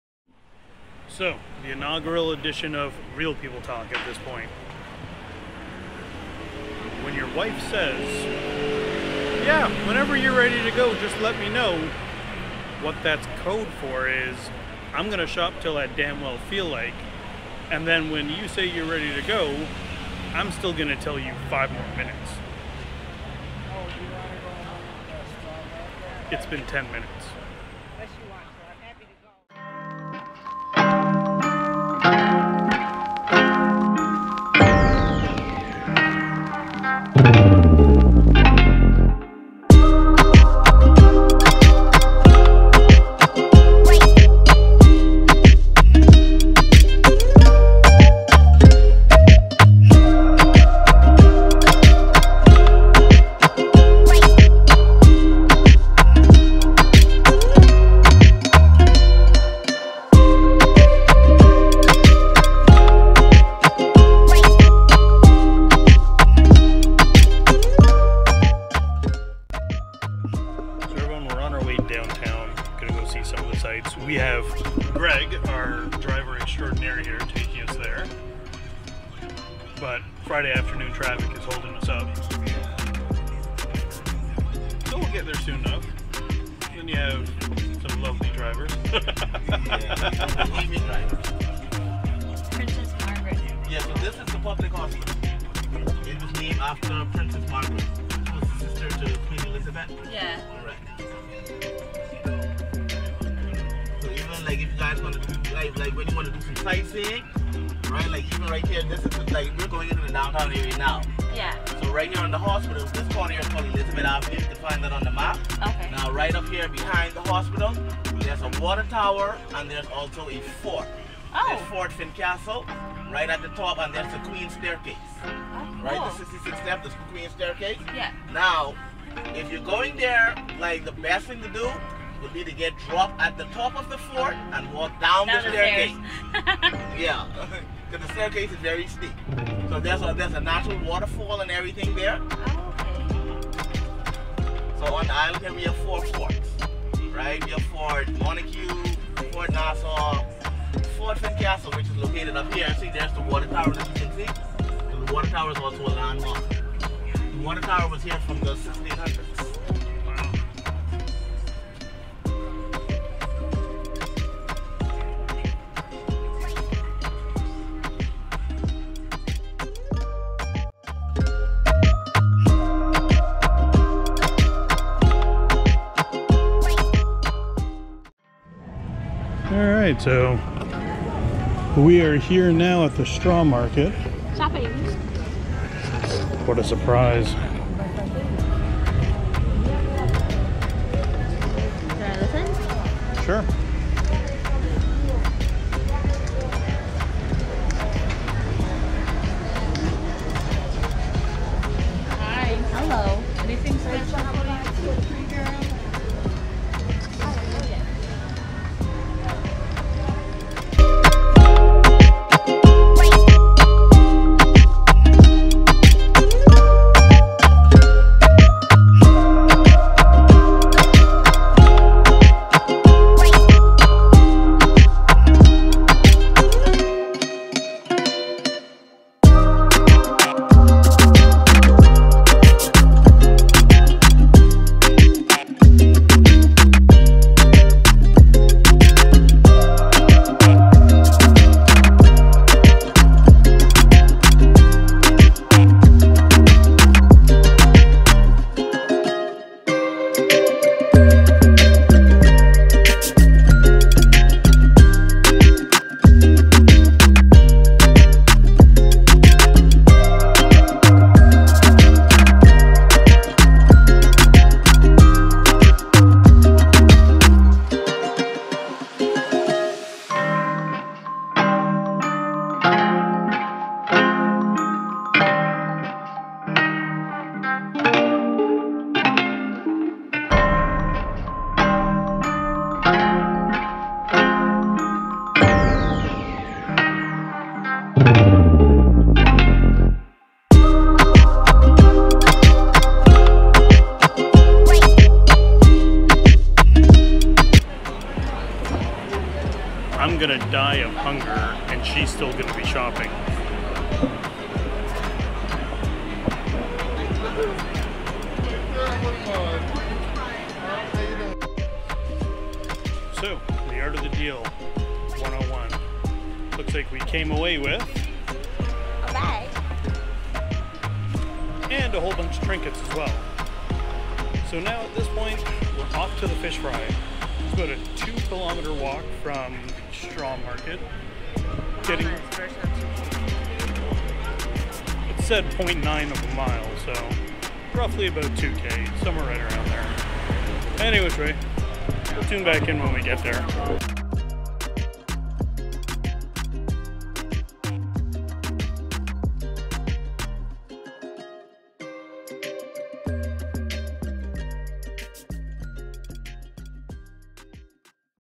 Vlog #3: Arrival Day - Atlantis Pt.3 - The Straw Market
3+ Hour Flight - Check
Drive through traffic to get to the resort - Check
Take a look a the room - Check
Off to do some shopping - Here we go!!

Flight From Westjet Vacations
Accommodations: Atlantis - The Royal @ Atlantis